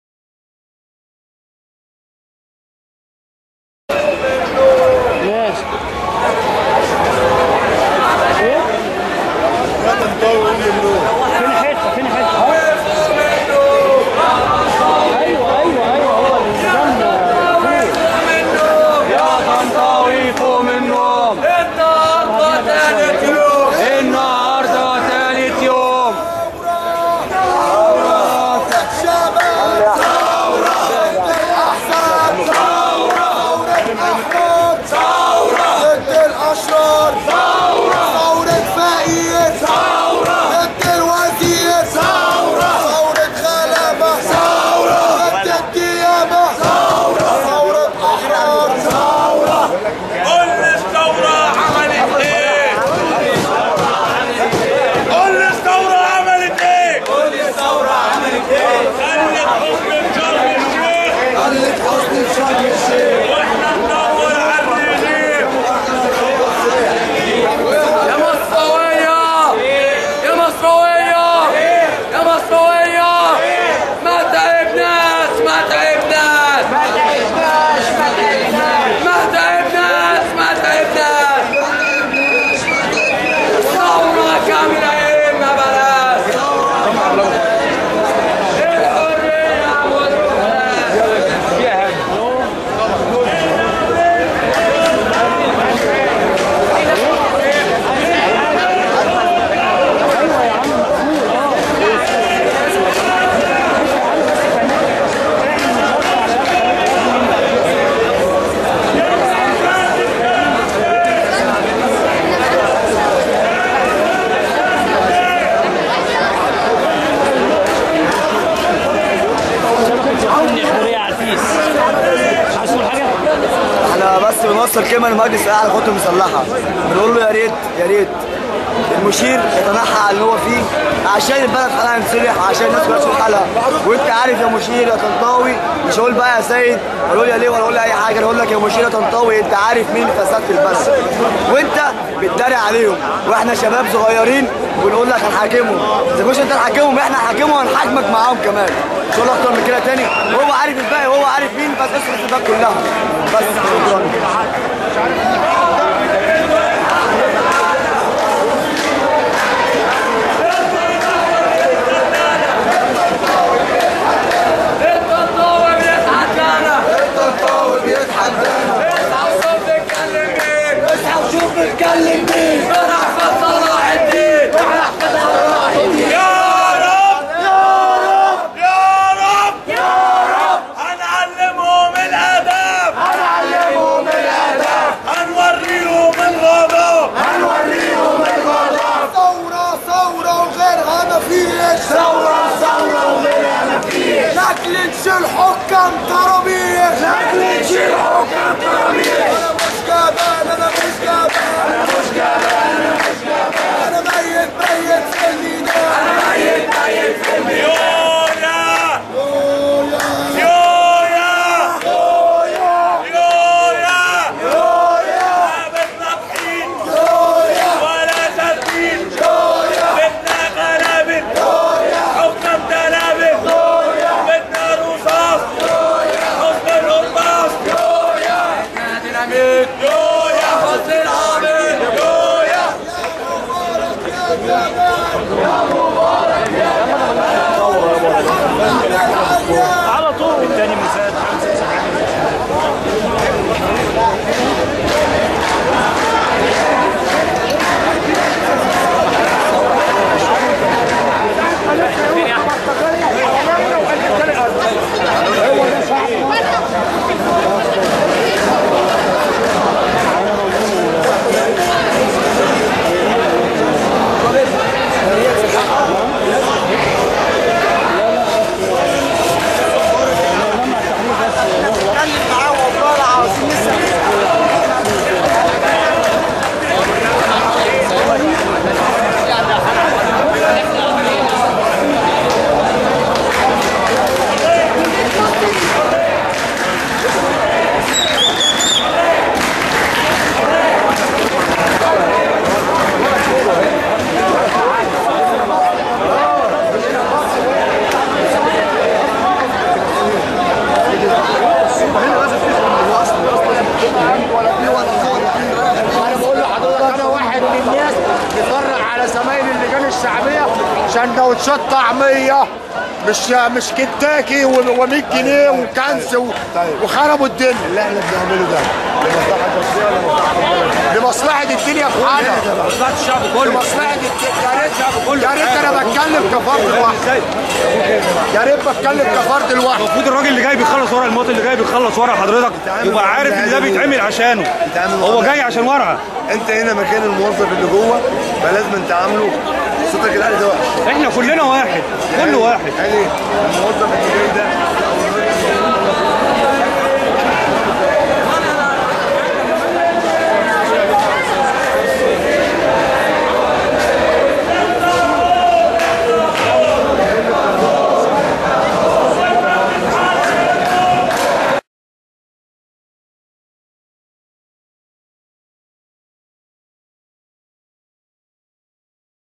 المعتصمون أمام مجمع التحرير يصرون على إغلاقه ج2
المعتصمون أمام مجمع التحرير يصرون على إغلاقه